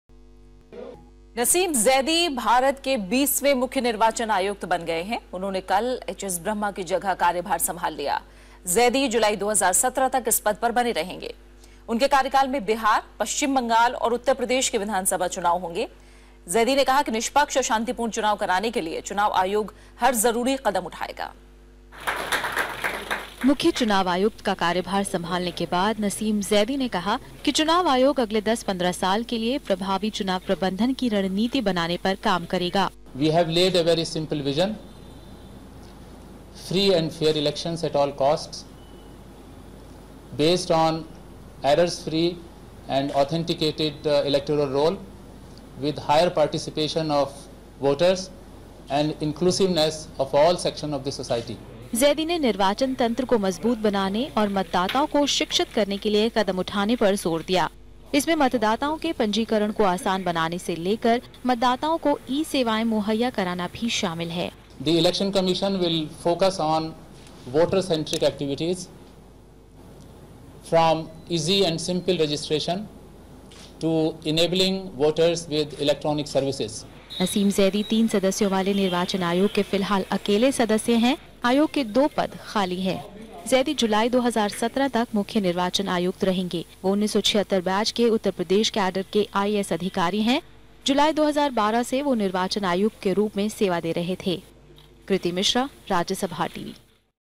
Nasim Zaidi takes over as new chief election commissioner
The new chief election commissioner, S Nasim Zaidi, on Sunday said the EC would soon devise a long-term strategic plan aimed at strengthening the institution, making the election operation more efficient and raising voter awareness.